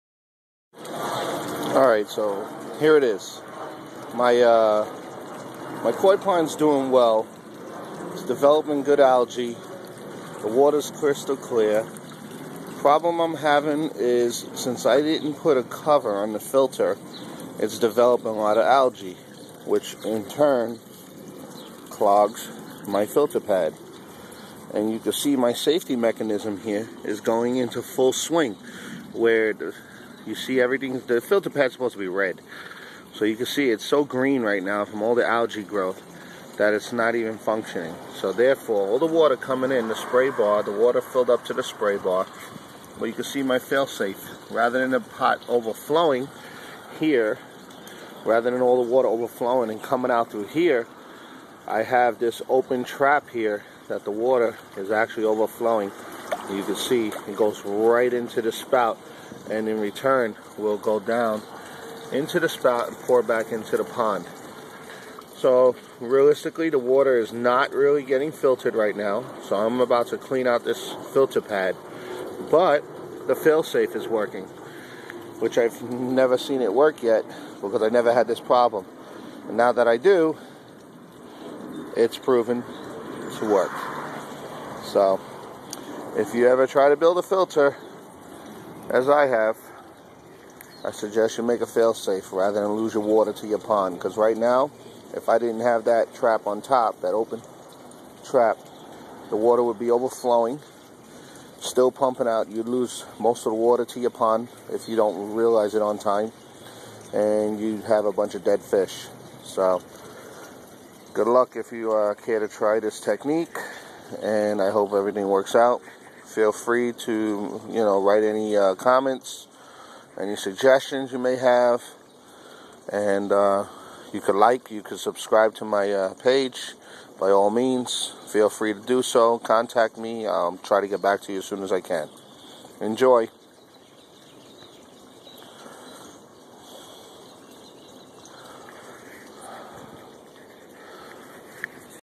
DIY filter
This is how the overflow saftey works.